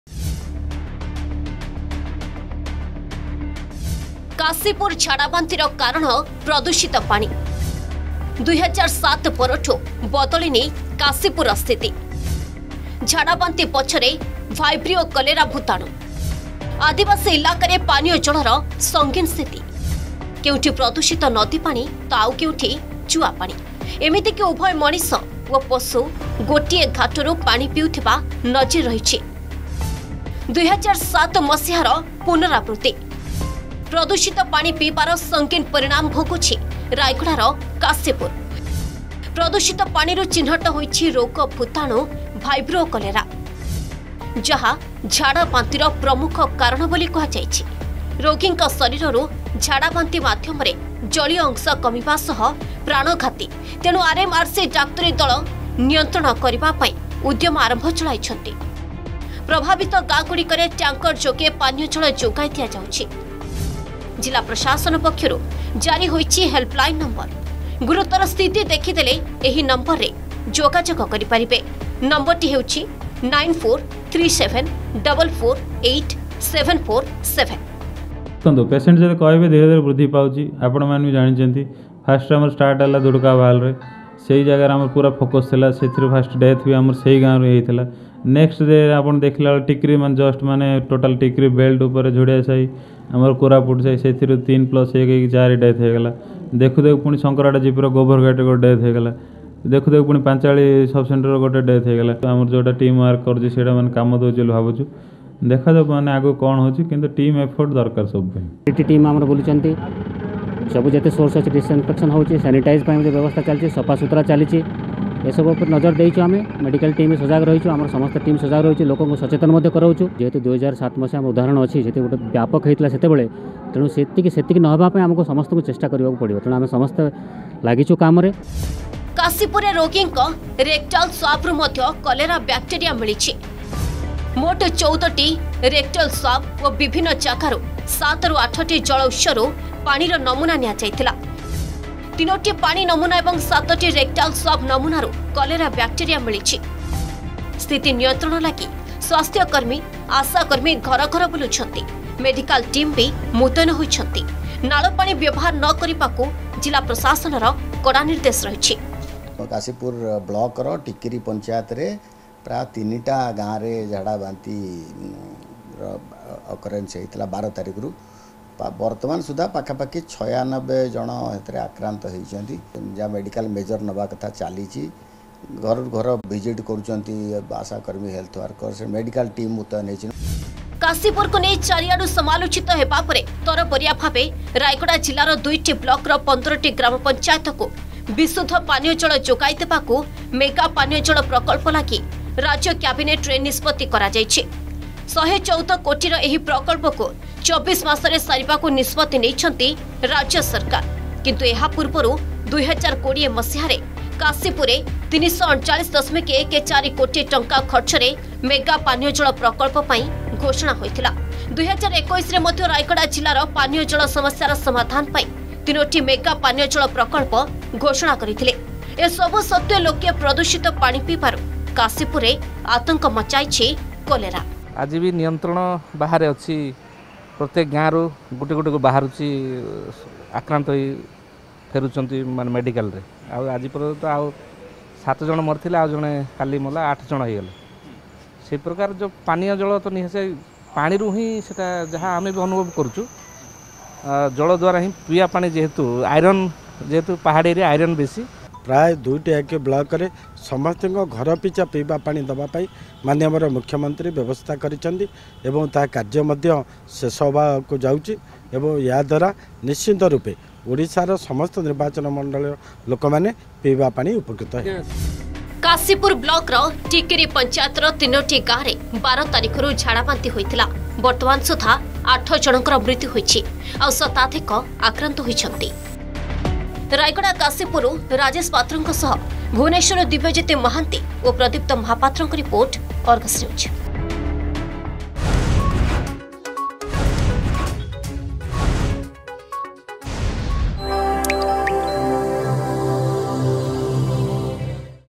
ଆଦିବାସୀ ଇଲାକାରେ ଜଳ ସାଜିଛି ଜହର
ଆଦିବାସୀ ଇଲାକାରେ ପାନୀୟ ଜଳର ସଙ୍ଗୀନ ସ୍ଥିତି। ପ୍ରଦୂଷିତ ପାଣିର ପରିଣାମ ଭୋଗୁଛି କାଶୀପୁର। ୨୦୦୭ର ହୋଇଛି ପୁନରାବୃତ୍ତି। ବ୍ଲକର ବିଭିନ୍ନ ଗାଁରେ କଲେରା ଭୂତାଣୁ ବ୍ୟାପୁଛି, ଗଲାଣି ୮ ଜୀବନ।